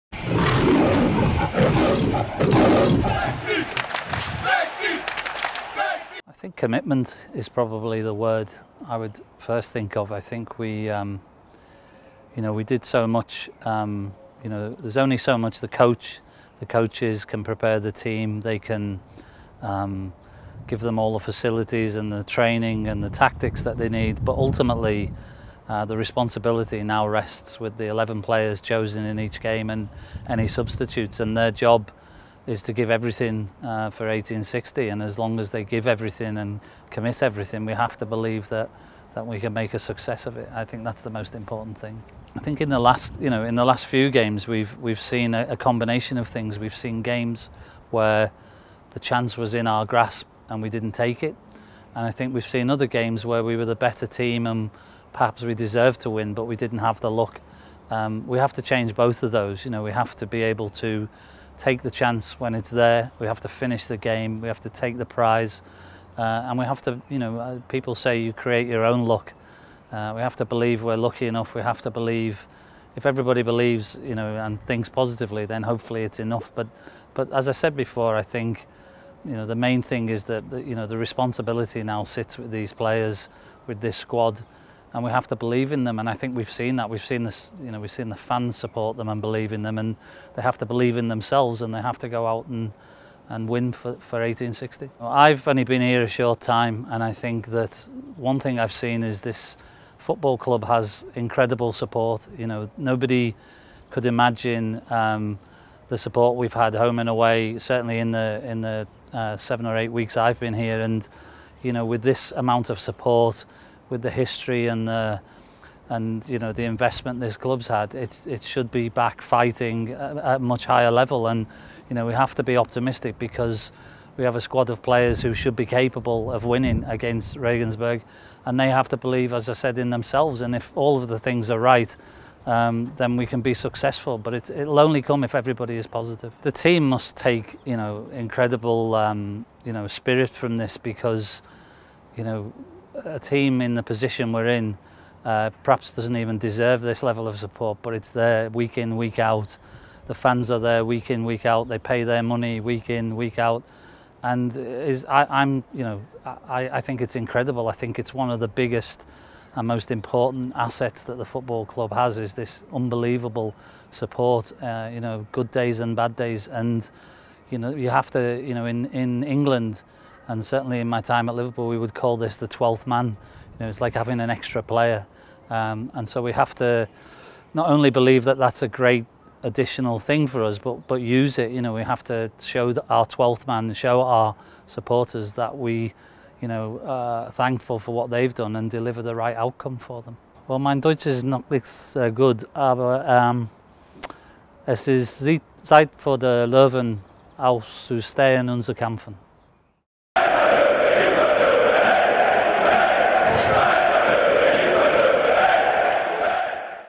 Löwenrunde // Ian Ayre: Alles für 1860 geben.
Vor dem Hinspiel in der Relegation gegen den SSV Jahn Regensburg spricht Löwen-Geschäftsführer Ian Ayre über die Bedeutung der zwei Finalspiele und nimmt die Mannschaft in die Pflicht.